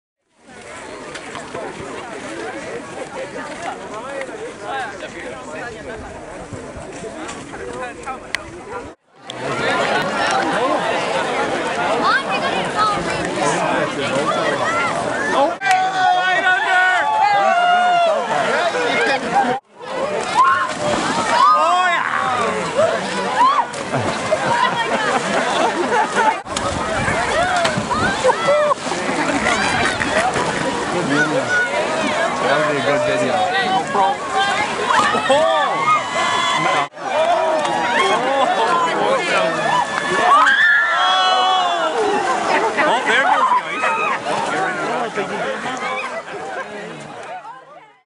Polar Bear Dip 2014
Hundreds lined the banks at the Charlottetown Yacht Club Jan. 1, 2014 to cheer on participants in the annual Polar Bear Swim. The harbour had already frozen over this year, so swimmers took turns to plunge into the icy water through a hole cut in the ice at the foot of Pownal Street.